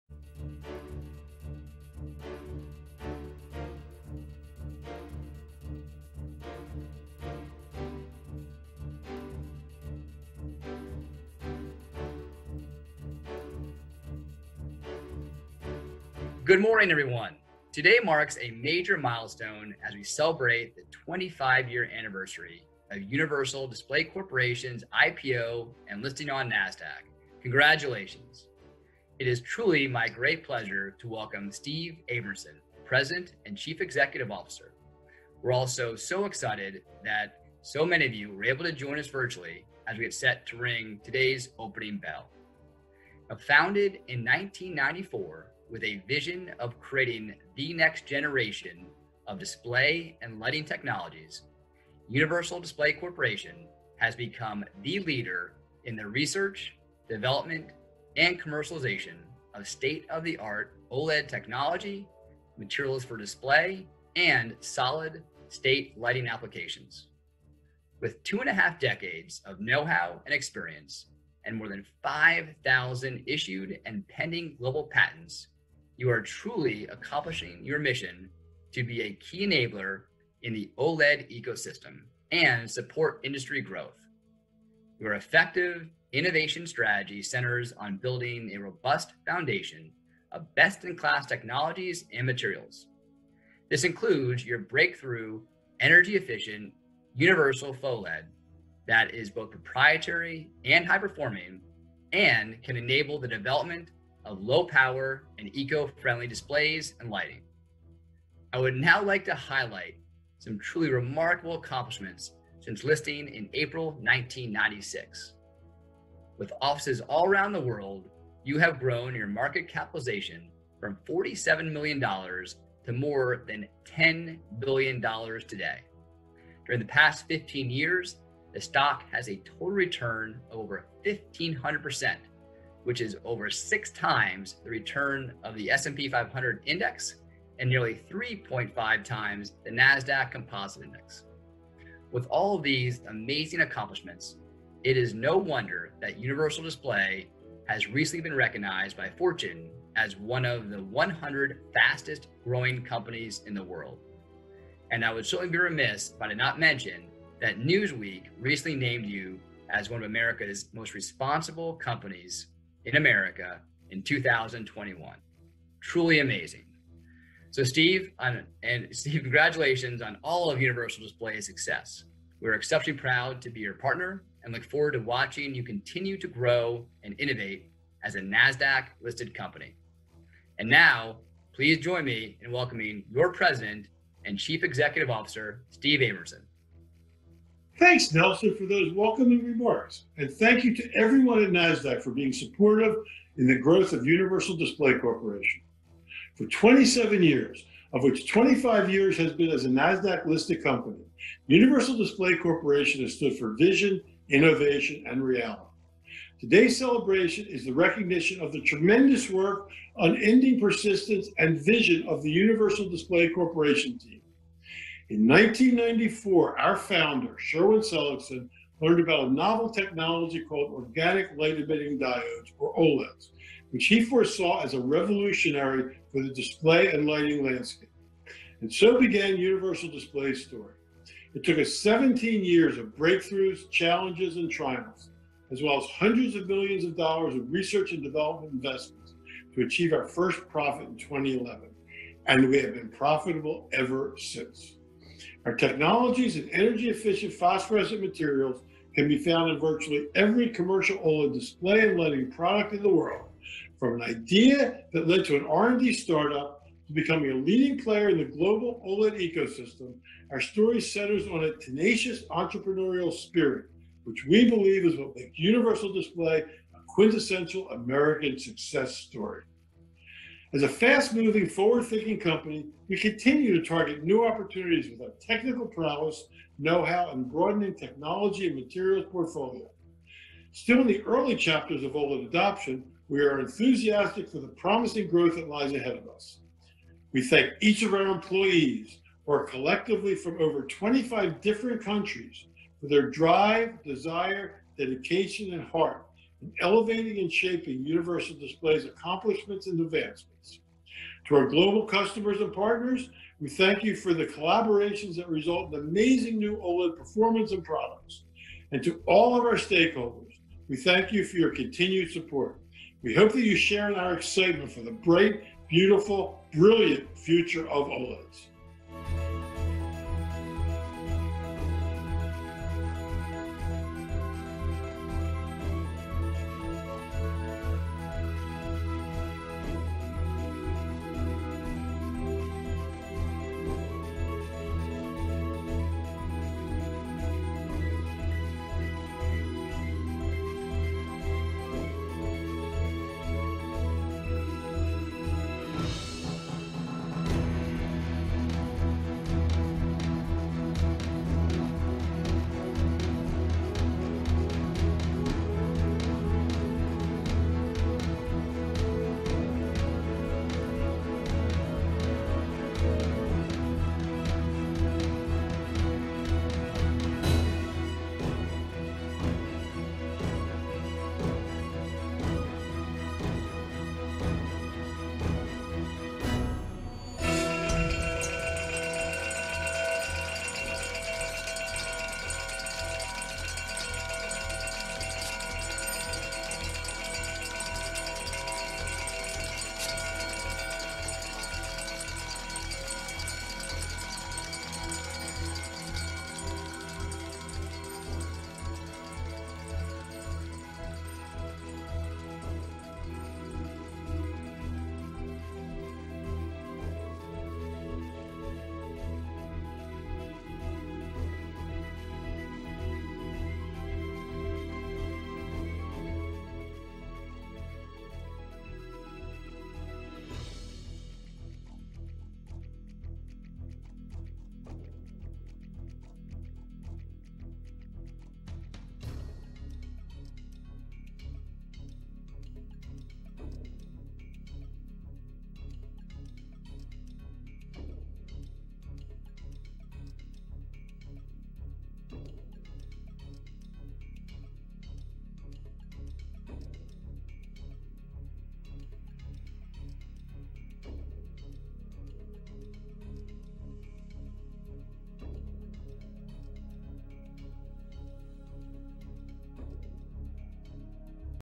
Universal Display Corporation Celebrating 25 Years as a Nasdaq-listed Company: Opening Bell Ceremony
Universal Display Corporation is thrilled to ring the @Nasdaq opening bell this morning (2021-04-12) and celebrate 25 years as a NASDAQ-listed company! Thanks to all our of all of our employees, partners, customers & shareholders!

"Today's celebration is the recognition of the tremendous work, unending persistence and 'Vision, Innovation and Reality' of the Universal Display Corporation team." Steve Abramson, President & CEO.